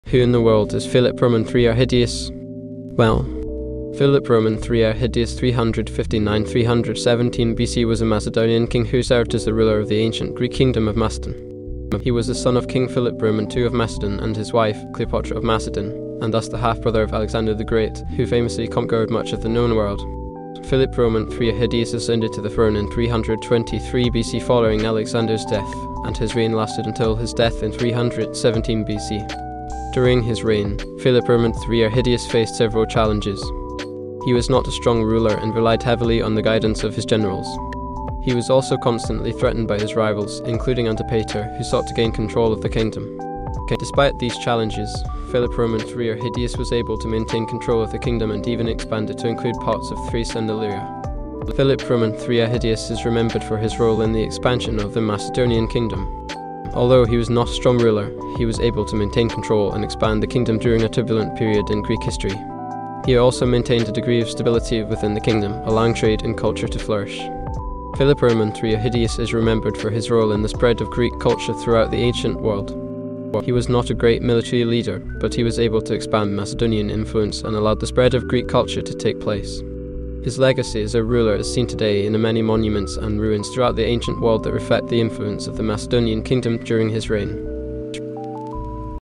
Philip III Arrhidaeus: A Short Animated Biographical Video
This biographical video captures the life and reign of Philip III Arrhidaeus, King of Macedon from 359 to 317 BCE.